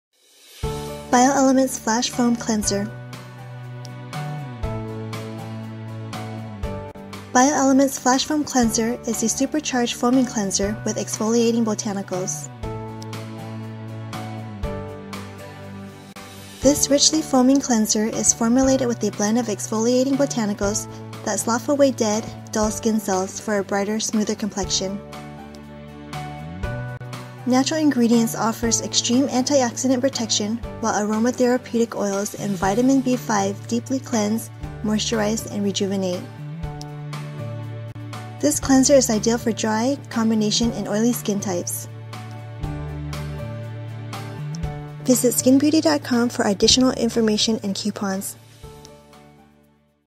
Bioelements Flash Foam Cleanser
Bioelements Flash Foam Cleanser smoothes and refines skin texture and is ideal for all Biotypes and skin conditions.